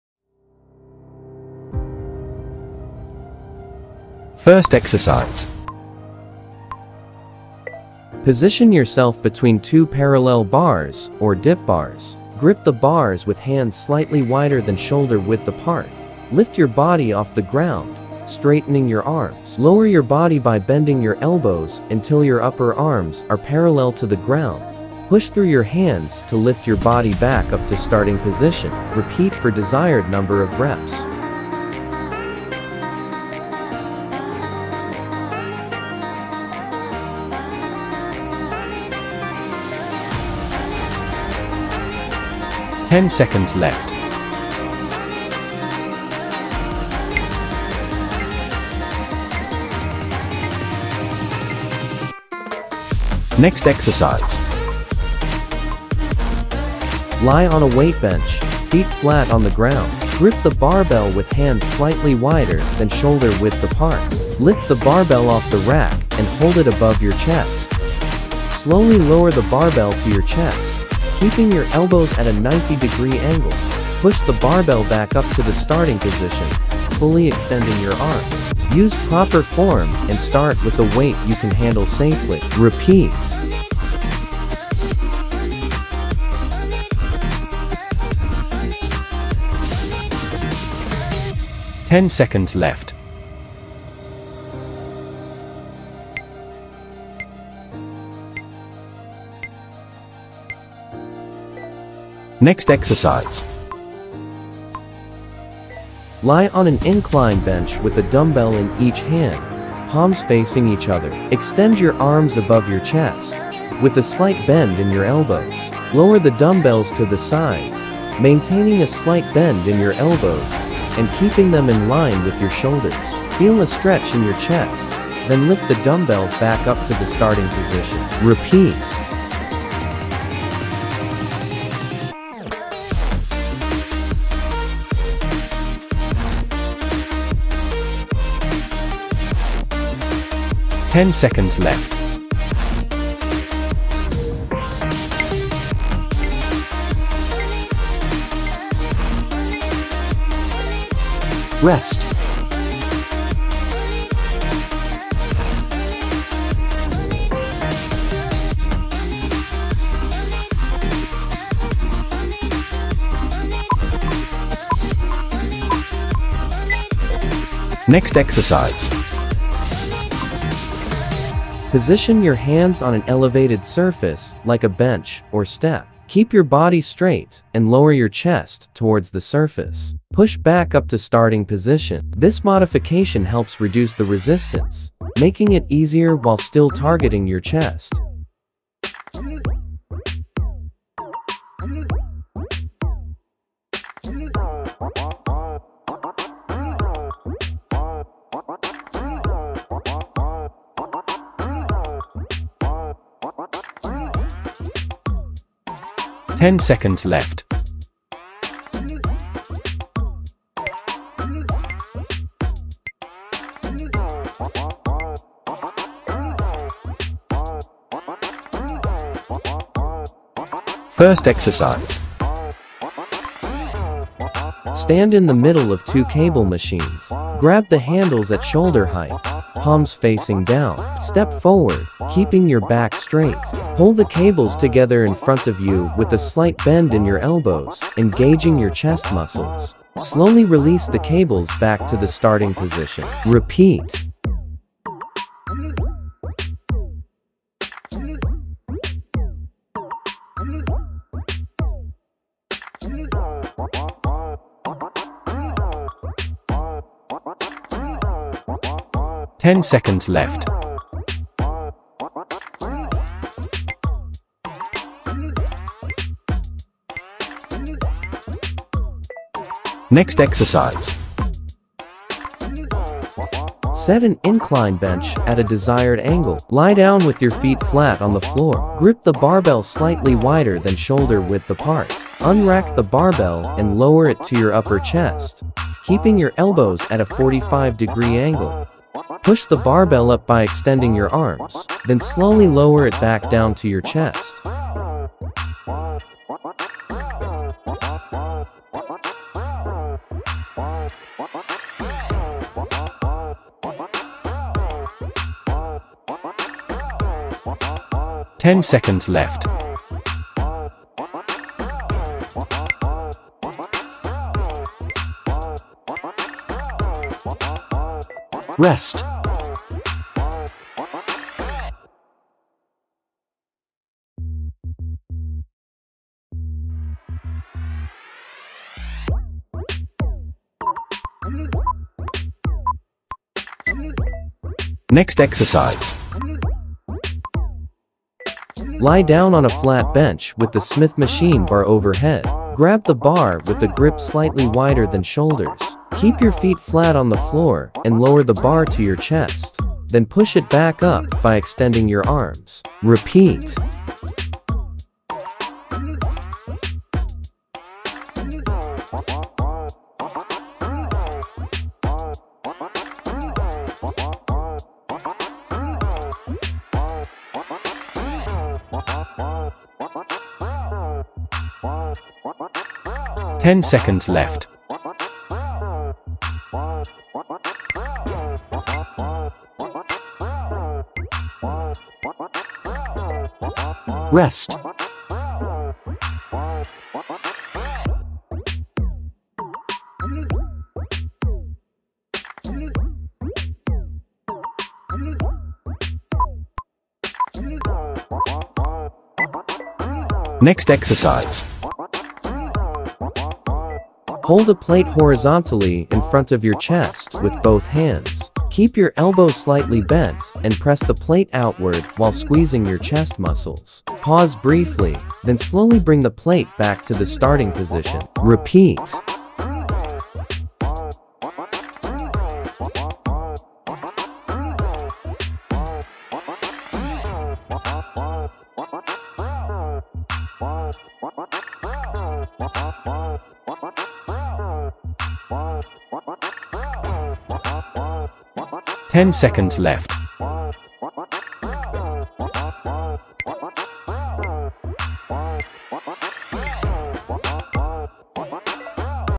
CAN'T GROW YOUR CHEST THIS 2-WEEK PLAN WILL HELP
Can't grow your chest here a 2 Weeks plan will help you get bigger chest